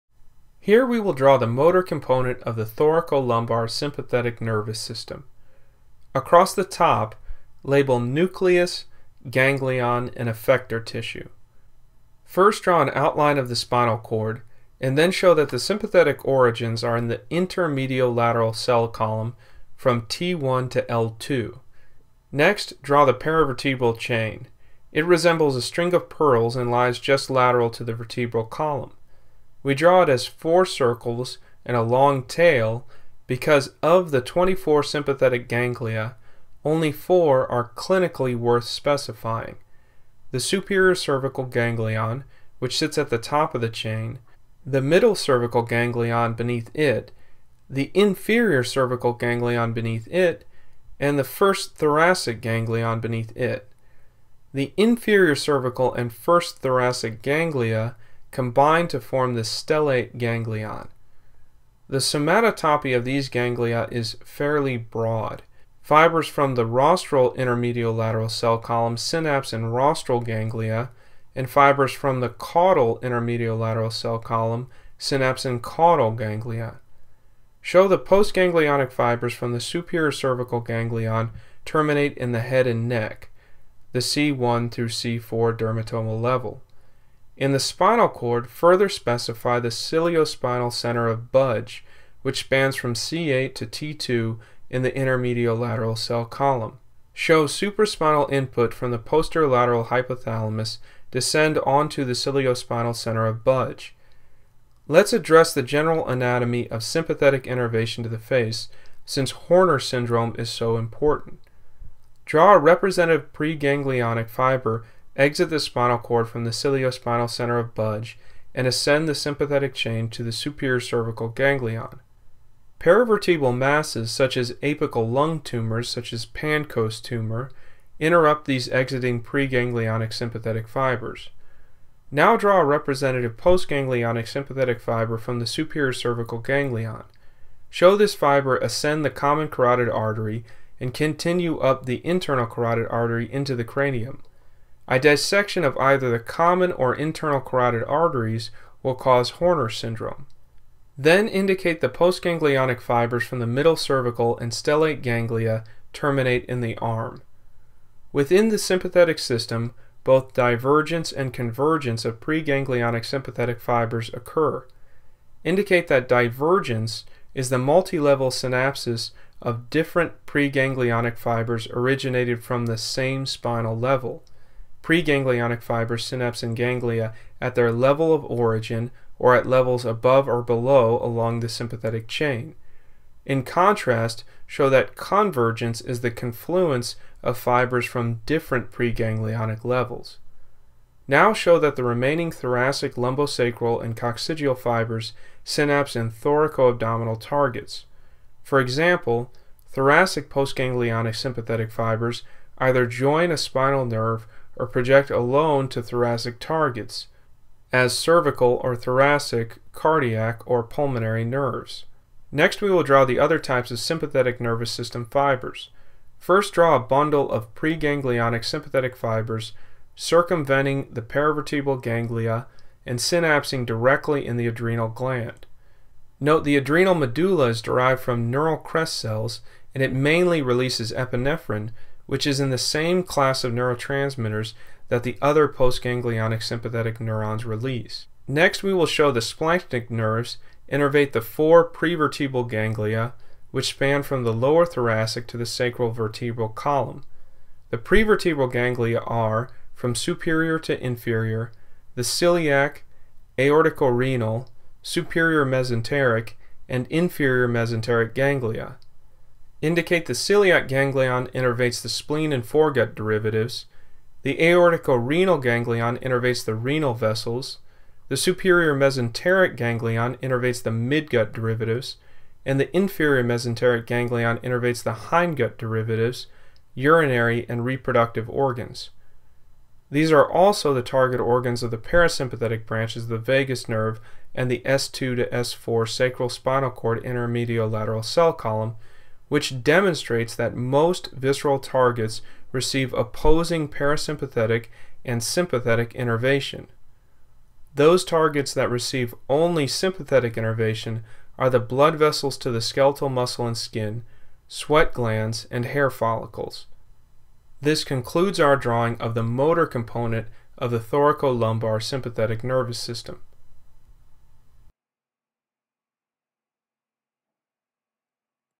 Sympathetic Nervous System - Draw it to Know it, Neuroanatomy
Learn the neuroanatomy of the sympathetic nervous system. Click the info button at the top right corner for links to our updated website and tutorials or find us in the app store!